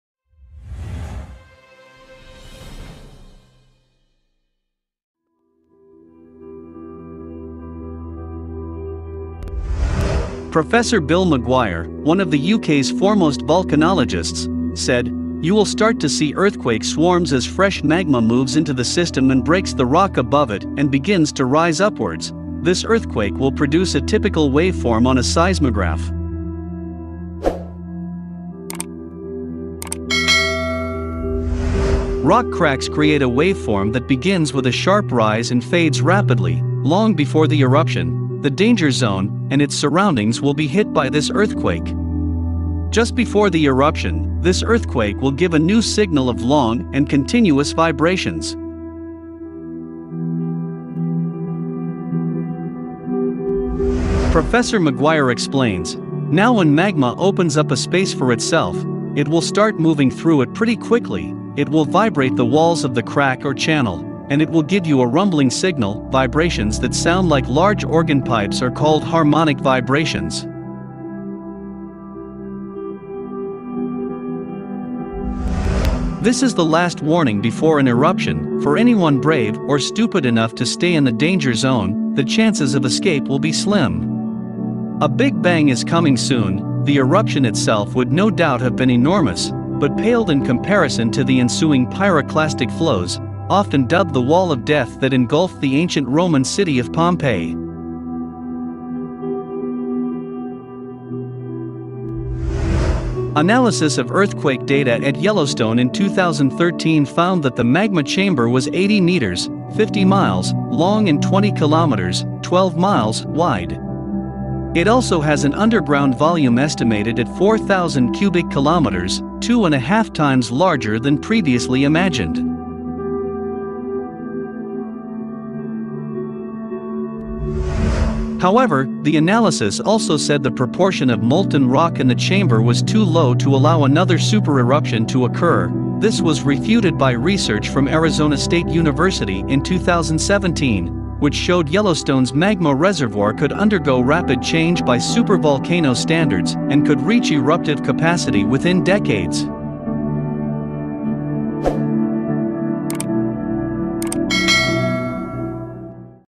Scientist Warn: one hour before sunset,as earth rises,Yellowstone will be hit by terrible earthquake
Professor Bill McGuire, one of the UK's foremost volcanologists, said: You will start to see earthquake swarms as fresh magma moves into the system and breaks the rock above it and begins to rise upwards,This earthquake will produce a typical waveform on a seismograph.
Rock cracks create a waveform that begins with a sharp rise and fades rapidly, Long before the eruption, the danger zone and its surroundings will be hit by this earthquake. Just before the eruption, this earthquake will give a new signal of long and continuous vibrations.
Prof McGuire explains: Now when magma opens up a space for itself, it will start moving through it pretty quickly, it will vibrate the walls of the crack or channel, and it will give you a rumbling signal, Vibrations that sound like large organ pipes are called harmonic vibrations.
This is the last warning before an eruption, For anyone brave or stupid enough to stay in the danger zone, the chances of escape will be slim. A big bang is coming soon, The eruption itself would no doubt have been enormous, but paled in comparison to the ensuing pyroclastic flows, often dubbed the "wall of death" that engulfed the ancient Roman city of Pompeii.